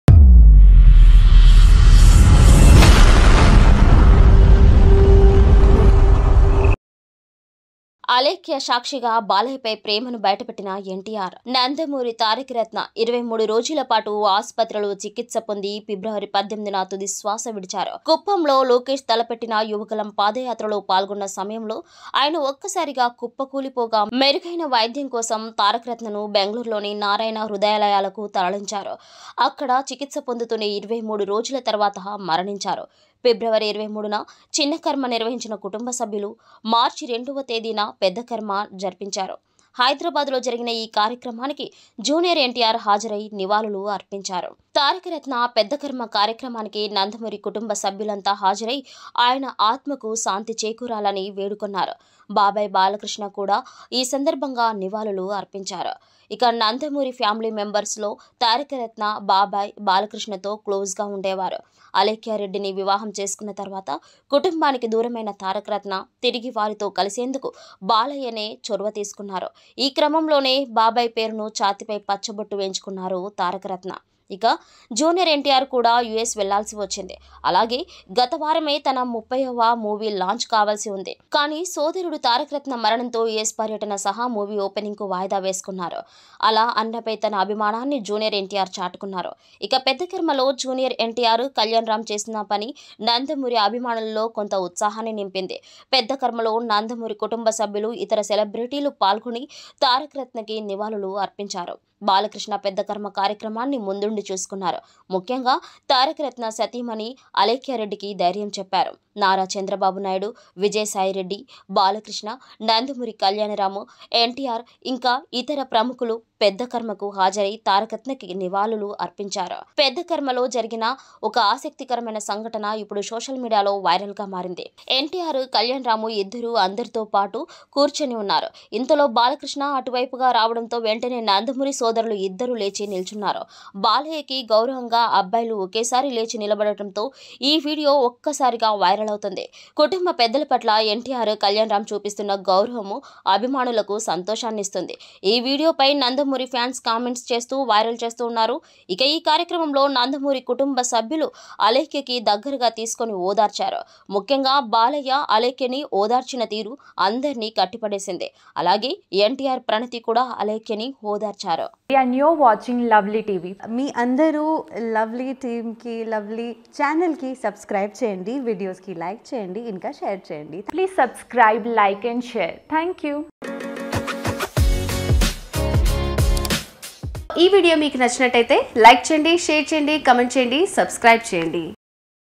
బాలయ్యపై ప్రేమని ఎన్టీఆర్ Alekhya Reddy on Taraka Ratna Pedda Karma, NTR Balakrishna Relation, Lovle
బాలయ్యపై ప్రేమని ఎన్టీఆర్ Alekhya Reddy on Taraka Ratna Pedda Karma, NTR Balakrishna Relation, Lovle Tv